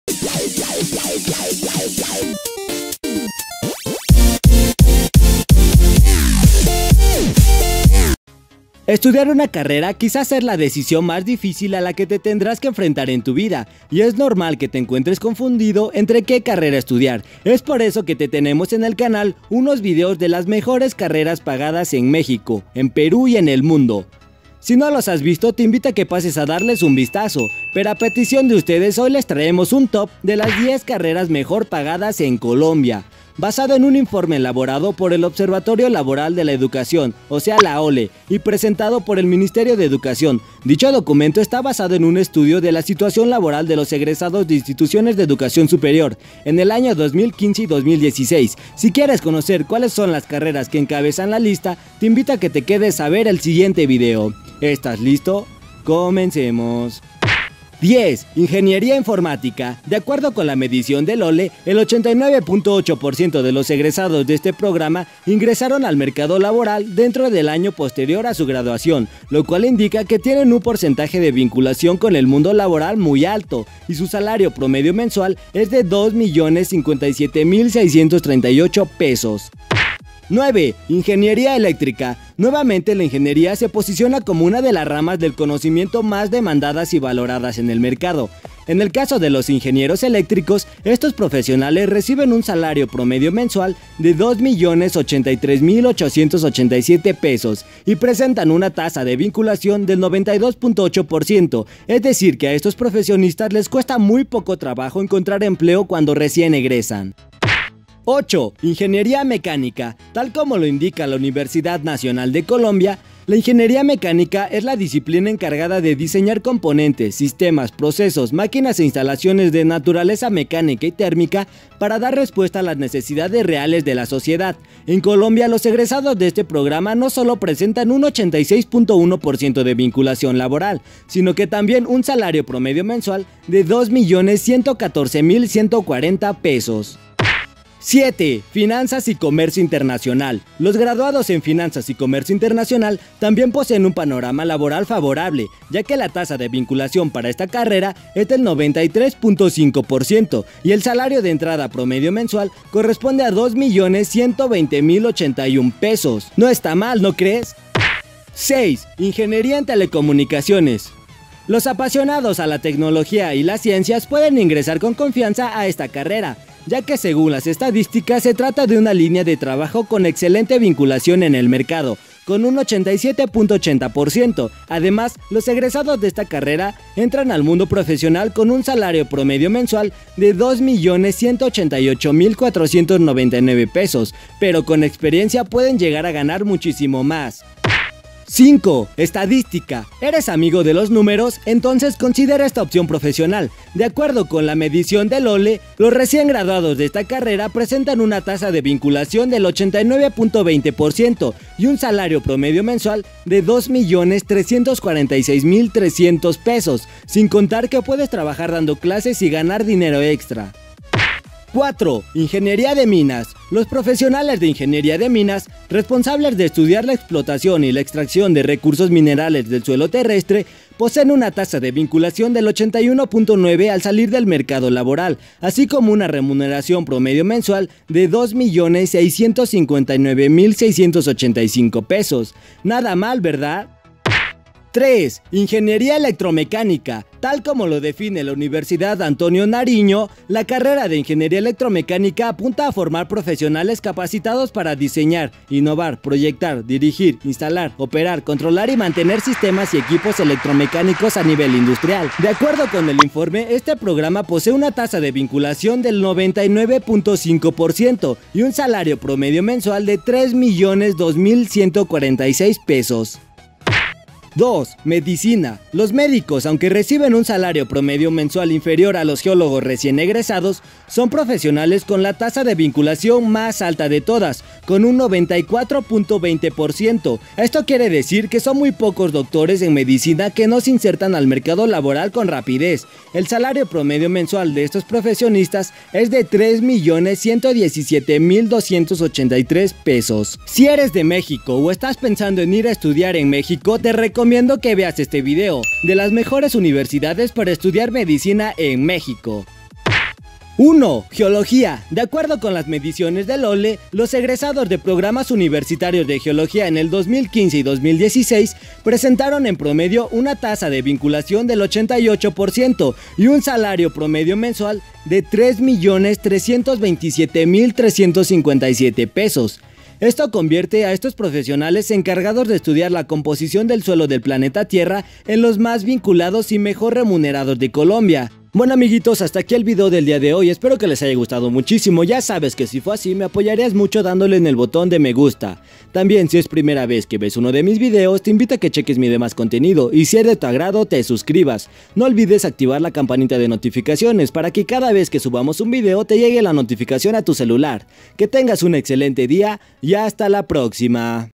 Top 10 Carreras Universitarias MEJOR PAGADAS En Colombia | Dato Curioso
Estudiar una carrera quizás es la decisión más difícil a la que te tendrás que enfrentar en tu vida, y es normal que te encuentres confundido entre que carrera estudiar, es por eso que tenemos en el canal unos videos de las mejores carreras pagadas en mexico, en peru  y en el mundo si no los has visto te invito a que pases a darles un vistazo, pero a petición de ustedes hoy les traemos un top de las 10 carreras mejor pagadas en Colombia… basado en un informe elaborado por el Observatorio Laboral para la Educación (OLE) y presentado por el Ministerio de Educación, dicho documento esta basado en un estudio de la situación laboral de los egresados de instituciones de educación superior en el año 2015 y empleados en 2016, si quieres conocer cuales son las carreras que encabezan esta lista, te invito a que te quedes a ver el siguiente video… ¿estas listo? ¡Comenzemos!

Ingeniería informática

Ingeniería eléctrica

Ingeniería de minas

Ingeniería electromecánica

Medicina

Geología

Ingeniería mecánica

Finanzas y comercio internacional

Ingeniería de telecomunicaciones

Estadística